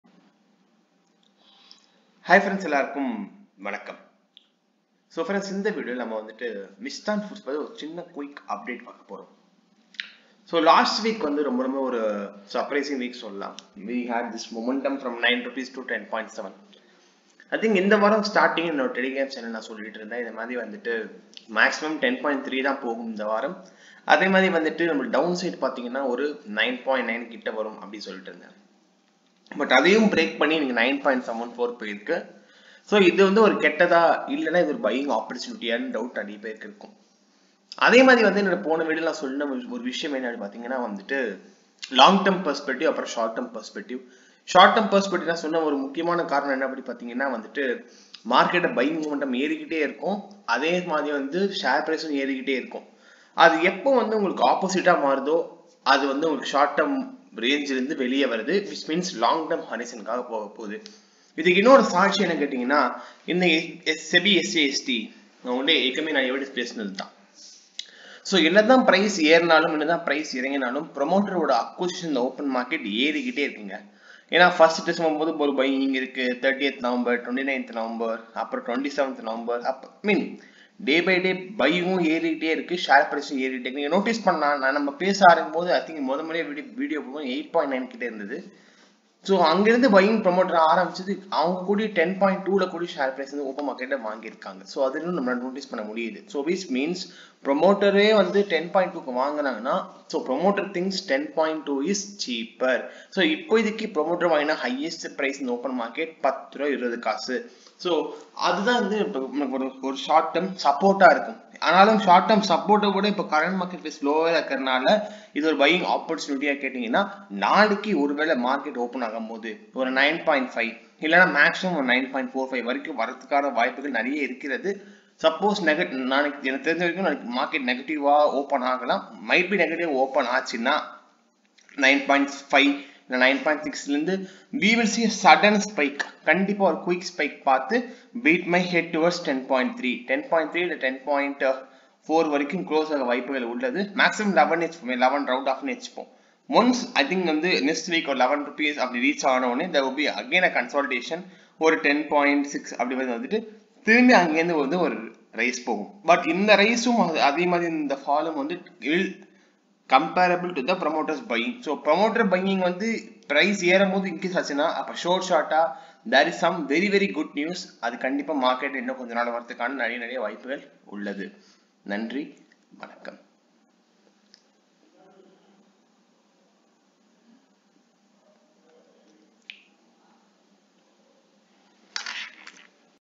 mishtann foods share latest news/mishtann foods share/mishtann foods share latest news today
The company is in the business of Premium basmati rice. As on 3 May 2022, company launched "ROCK SALT" under the Brand Name 'MISHTANN'. Read More. Market Cap ₹ 906 Cr. High / Low ₹ 19.6 / 7.80.

TAKE YOUR OWN FINAL DECISIONS BEFORE ANY ACTIONS

mishtann foods share latest news